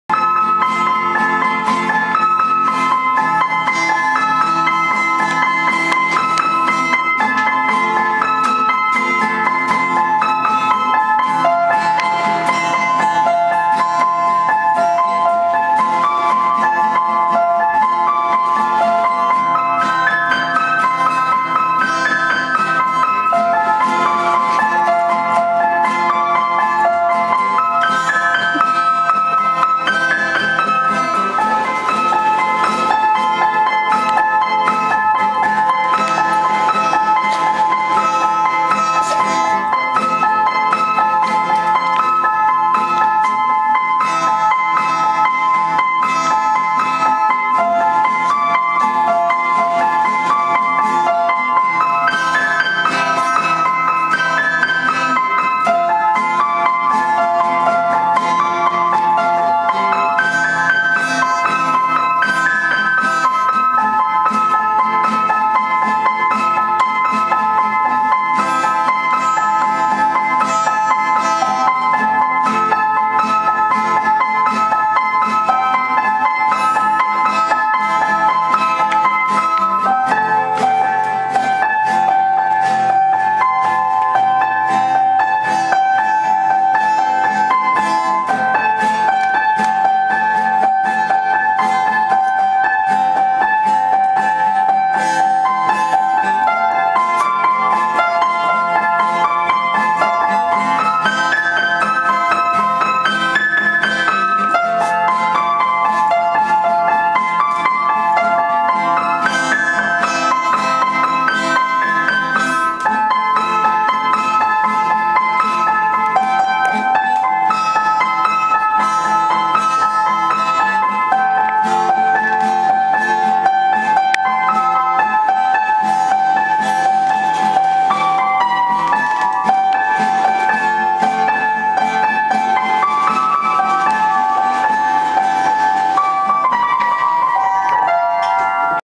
typical day at my house...
always music going on :)
my brother on piano and my dad on guitar just relaxing. oh yea and snowball my dog/son prancing around and my mom on the sewing machine in the background hah.